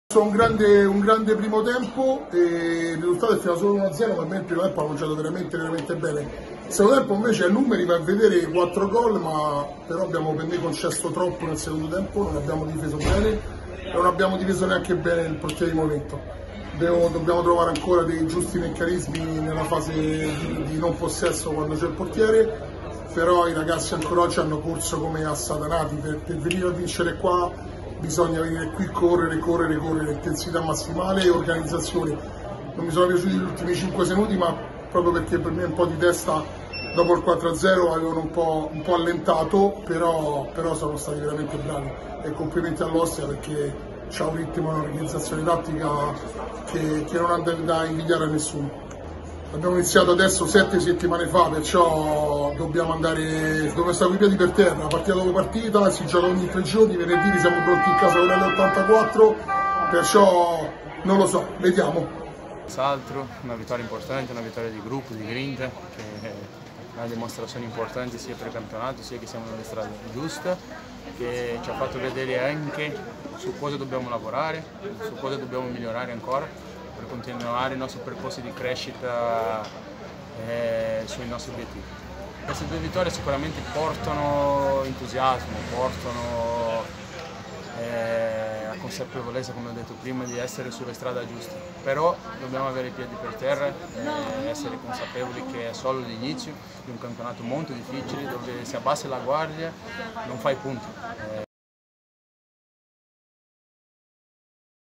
Sandro Abate, Angelini e Perez suonano la carica
"E' solo l'inizio dobbiamo rimanere con i piedi ben saldi a terra": così i due dopo la vittoria arrivata contro il Todis Lido di Ostia nel turno infrasettimanale